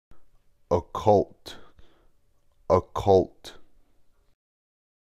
How to Pronounce OCCULT
occult

adjective

1.
Of, relating to, or dealing with supernatural or magical influences, agencies, or occurrences.

2.
Available only to the initiate; secret or mysterious: synonym: mysterious.
"occult lore."

3.
Beyond the realm of human comprehension; inscrutable.